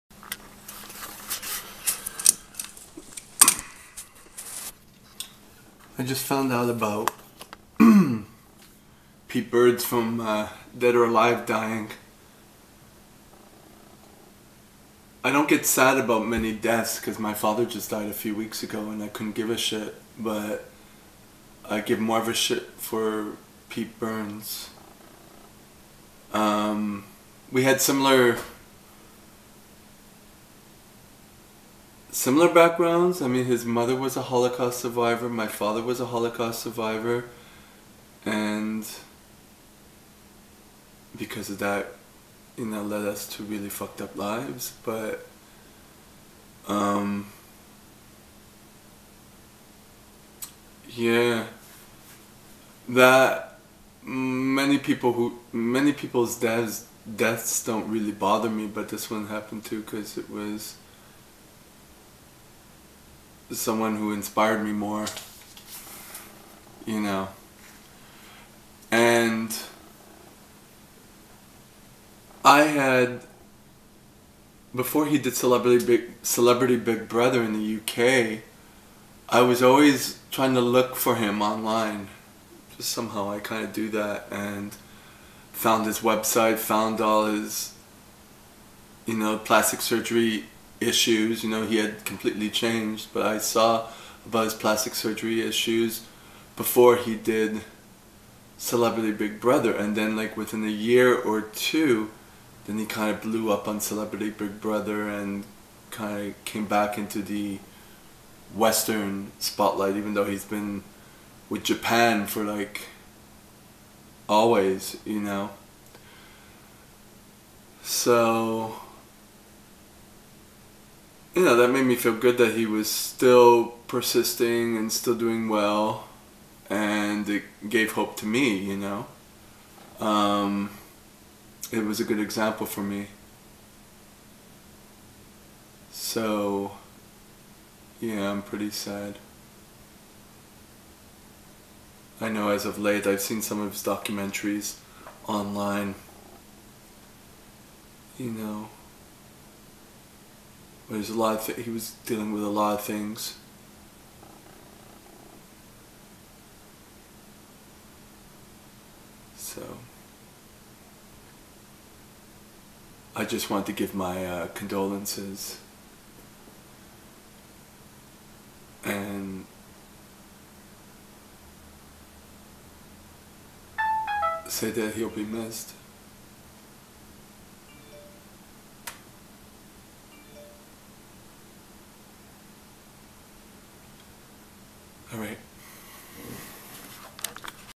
Passing of Pete Burns from Dead or Alive
I'm deeply saddened of the passing of Pete Burns from Dead or Alive